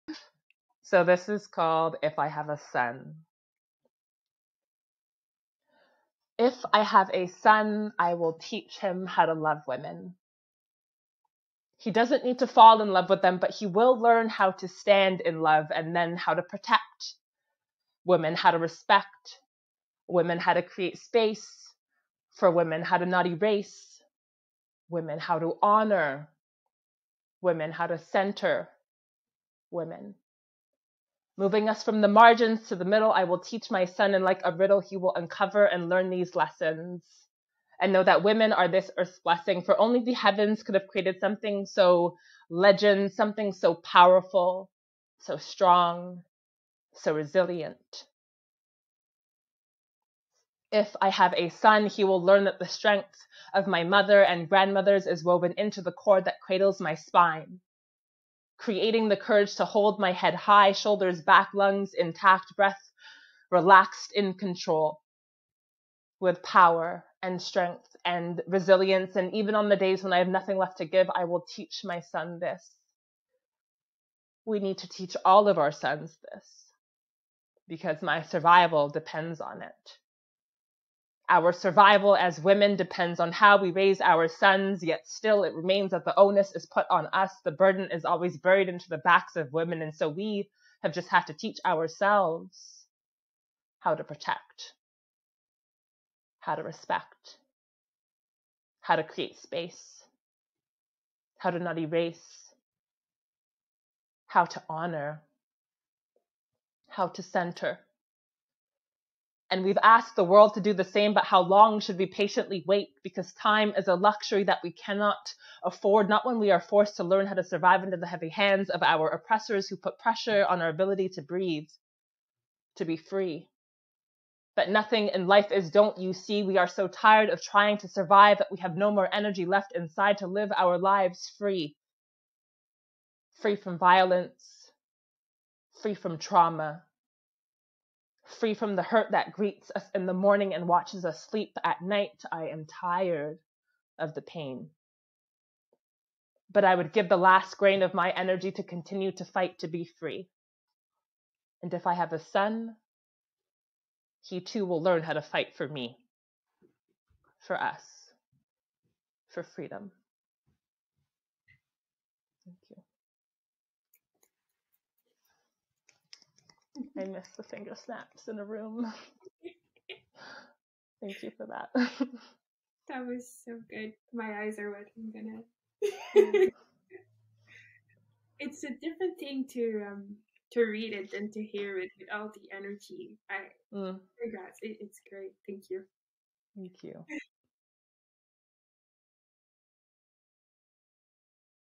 "If I Have a Son" Spoken word artist Shanice Nicole performs her poem
Now that she has a newborn son, Dear Black Girls author Shanice Nicole sees this poem she wrote years ago in new light. Enjoy this performance at any age. *Finger snaps*

If I Have A Son
by Shanice Nicole

I will teach him how to love women.
He doesn’t need to fall in love with them
but he will learn how to stand in love
and then
how to protect women,
how to respect women,
how to create space for women,
how to not erase women,
how to honour women,
how to centre women
moving us from the margins to the middle
I will teach my son
and like a riddle
he will uncover and learn these lessons
and know that women are this Earth’s blessing
for only the heavens could have created something so legend
something so powerful,
so strong,
so resilient.

he will learn that the strength of my mother and grandmothers
is woven into the cord that cradles my spine
creating the courage to hold my head high,
shoulders back,
lungs intact,
breath relaxed
in control
with power,
and strength
and resilience
even on the days when I have nothing left to give,
I will teach my son this.

We need to teach all of our sons this.
because my survival depends on it.
Our survival as women depends on how we raise our sons
yet still, it remains that the onus is put on us
the burden is always buried into the backs of women and so
we have had no choice but to teach ourselves
how to protect,
how to respect,
how to create space,
how to not erase,
how to honour,
how to centre.
And we’ve asked the world to do the same
but how long should we patiently wait
because time is a luxury we cannot afford
not when we are forced
to learn how to survive
under the hands of our oppressors,
who put pressure on our ability
to breathe,
to be free,
but nothing in life is

We are so tired of trying to survive
that we have no more energy left inside
to live our lives
free.
Free from violence.
Free from trauma.
Free from the hurt that greets us in the morning
and watches us sleep at night
I am tired of the pain,
but I would give the last grain of my energy
to continue to fight to be free.
And if I have a son,
he too will learn how to fight for me.
For us.
For freedom.